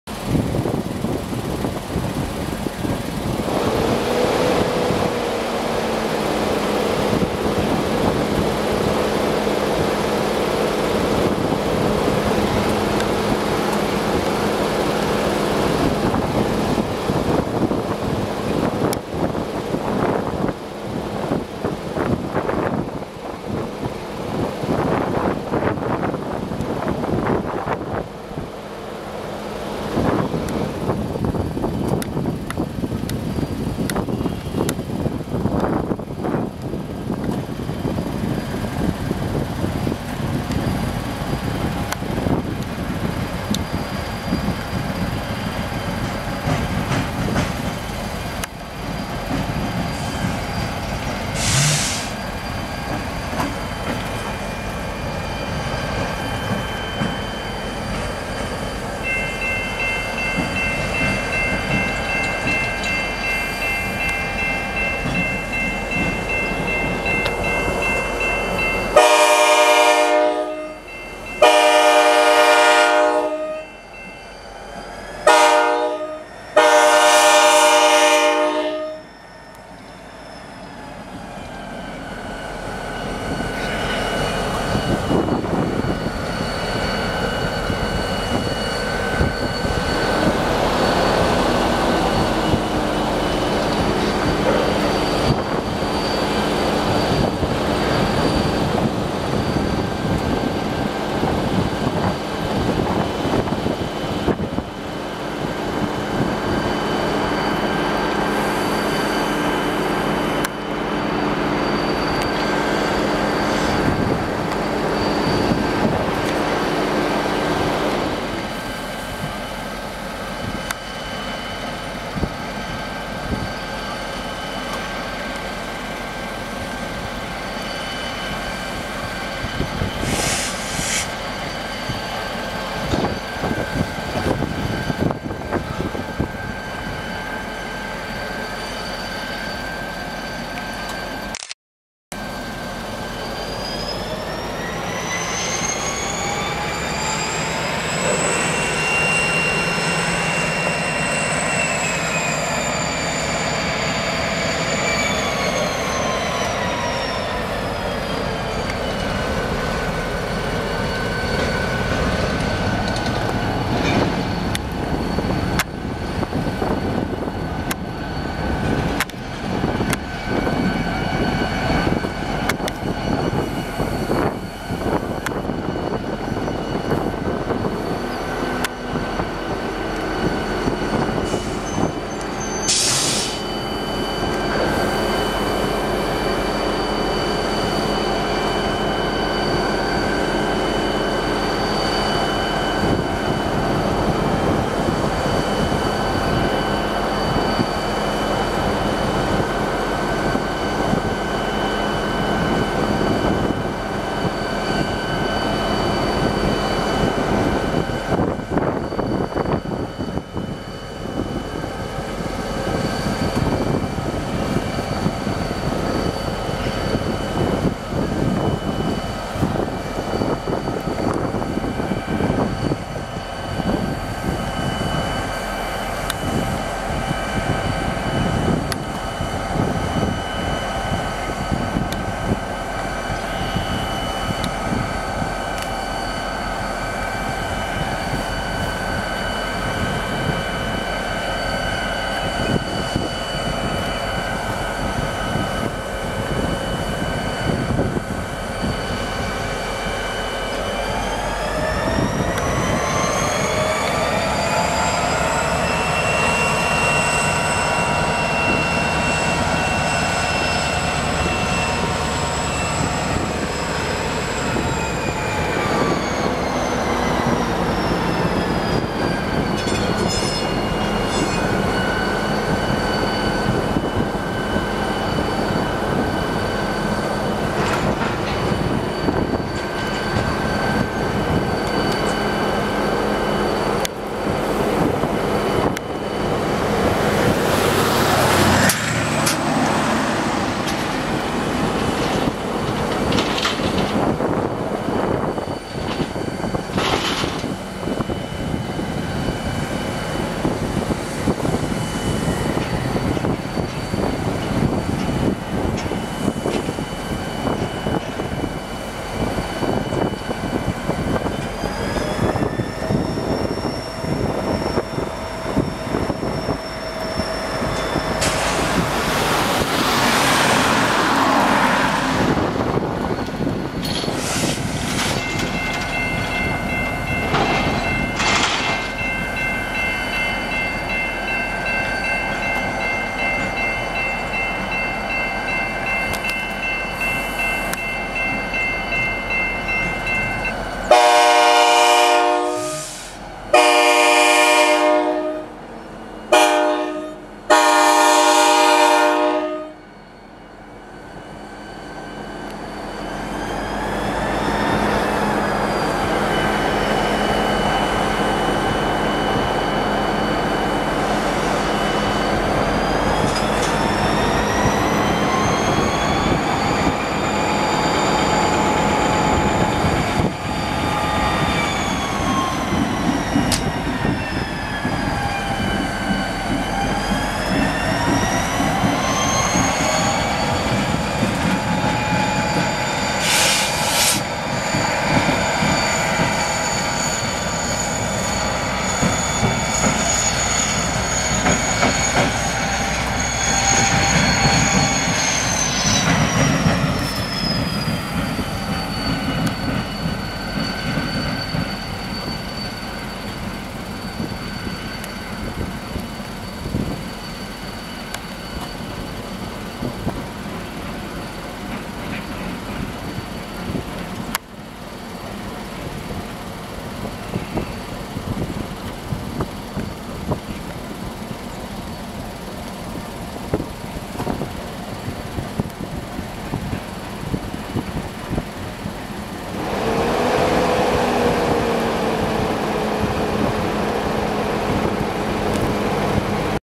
Ex LEASER NS SD40-2 3570 Switching in Fostoria, OH
Video was mostly taken in place of Photos for Modeling purpose